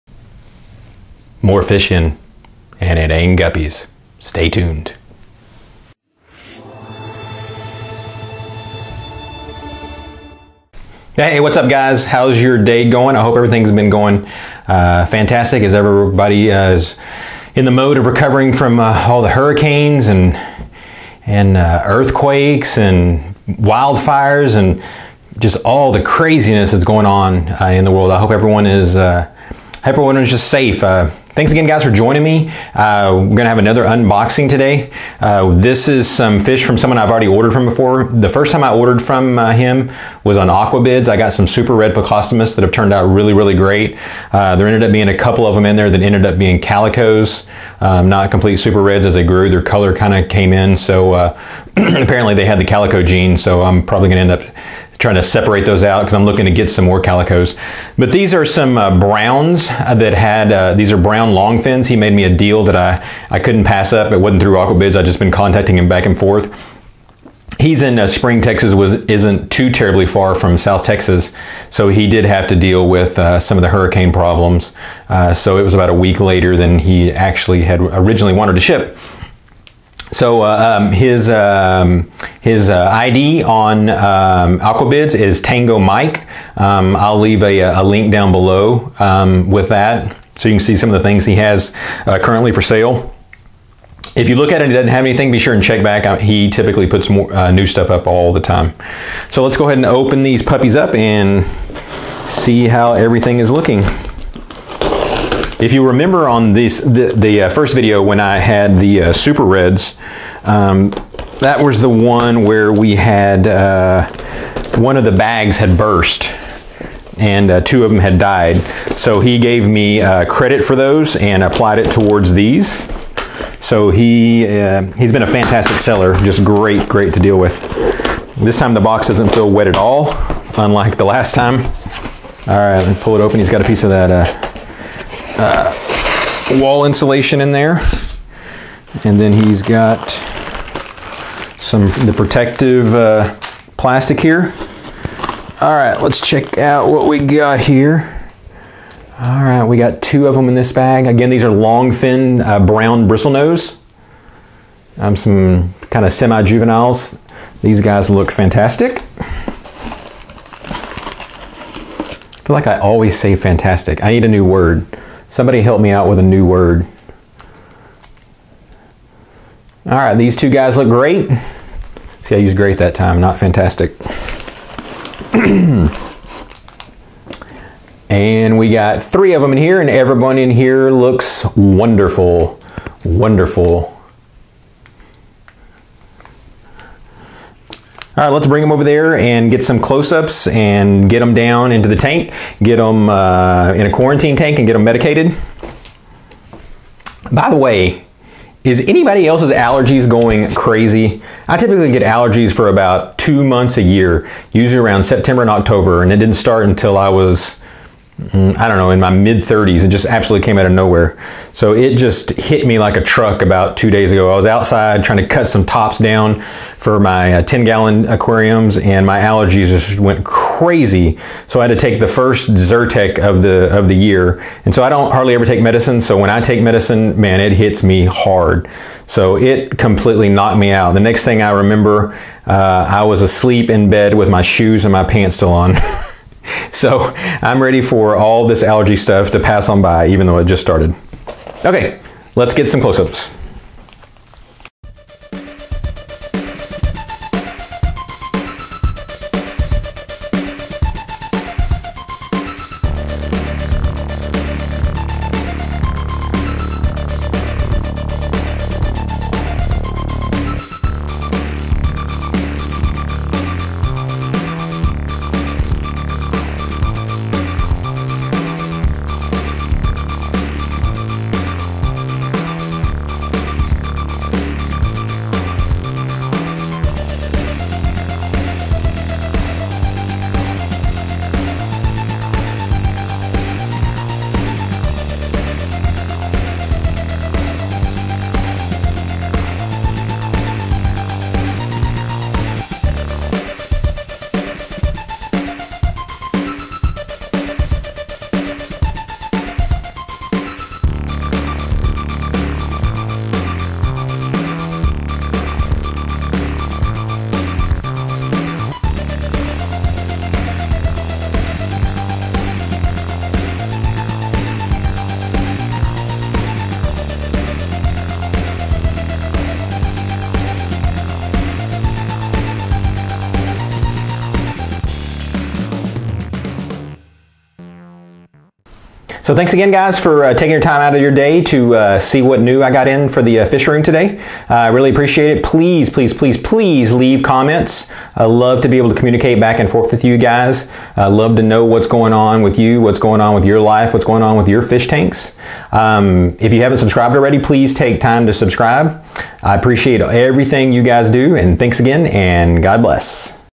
Un-boxing Chocolate Brown Longfin Pleco in the Fish Room
Another Fishroom unboxing. Longfin Chocolate Brown Plecostomus. Adding more awesome fish to the Fish Room.

Wanna Help Me Out and Get Free Stuff!!

Camera
Canon SL2

Mic
Rhode VideoMic Go

Lighting System

Editing Software
Filmora